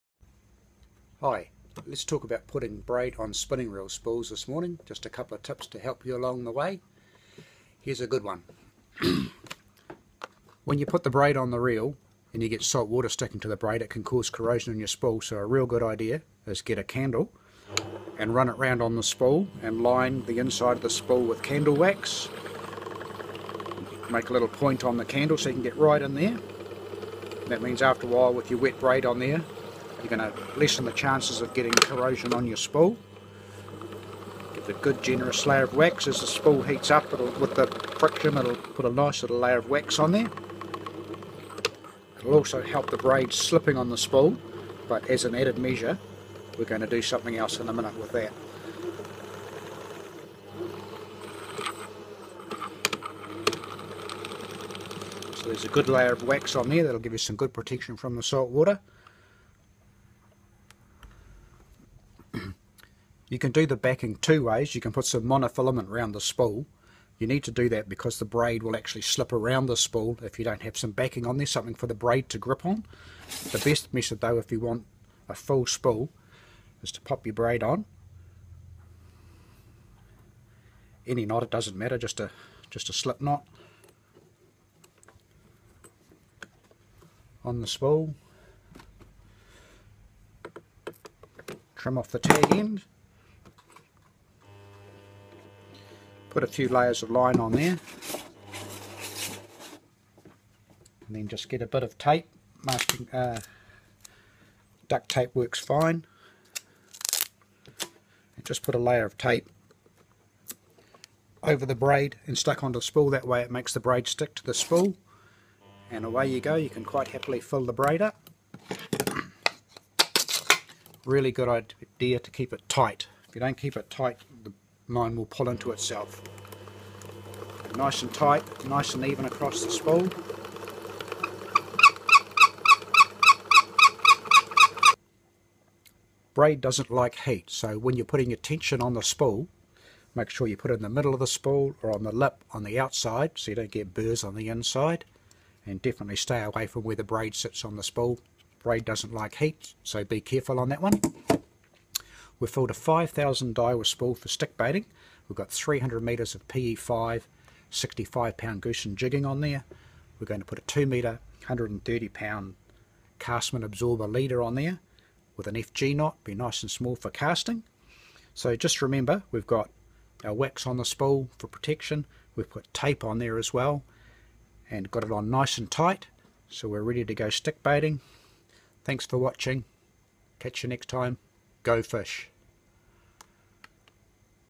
Spooling a spinning reel with braid fishing line
Helpful tips when filling up a spin reel with PE braided line. Backing the braid and making sure the braid doesn't slip on the spool.